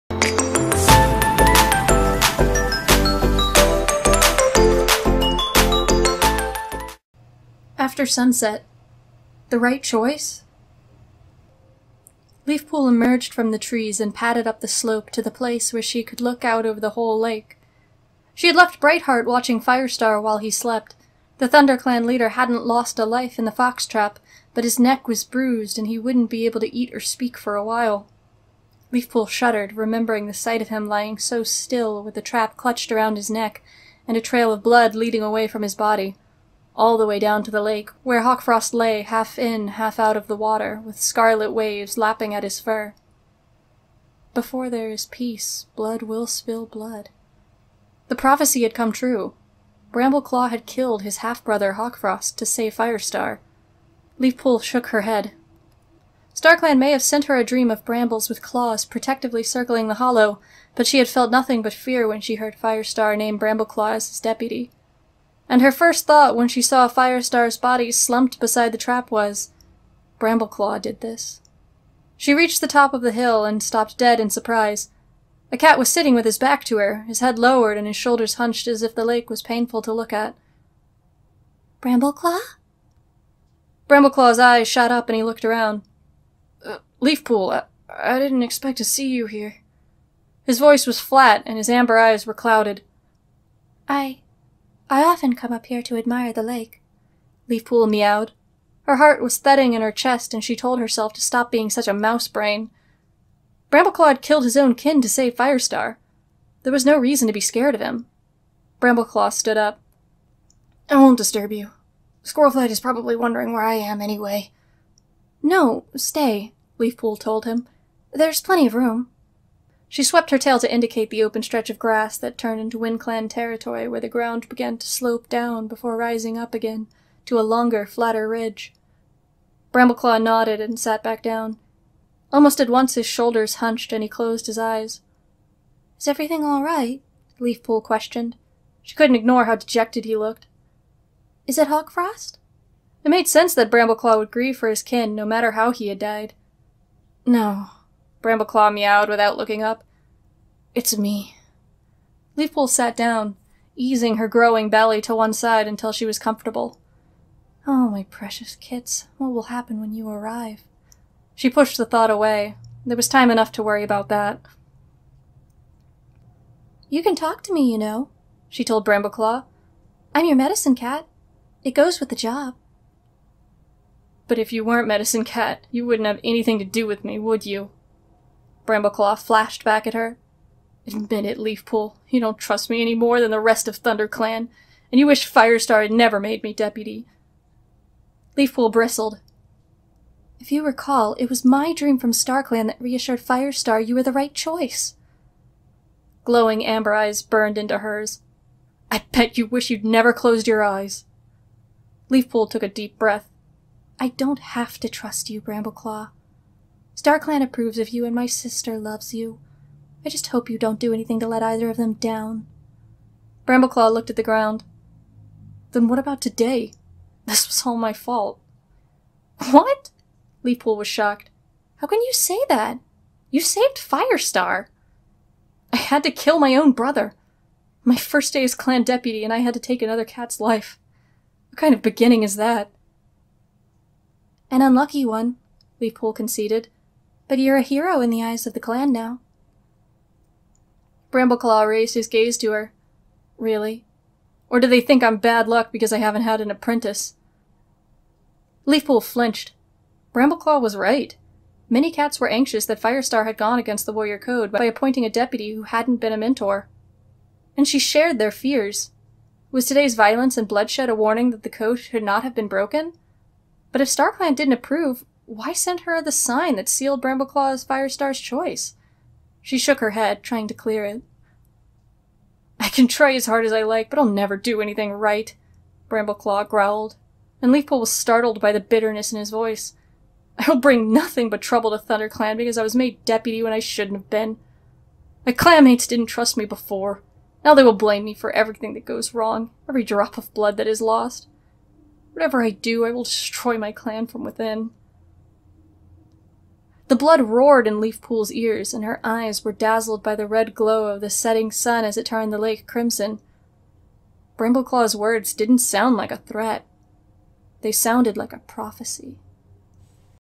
Nate Reads Warrior Cats: After Sunset | The Right Choice?
A little canon short story! I believe this is from the old Warriors App?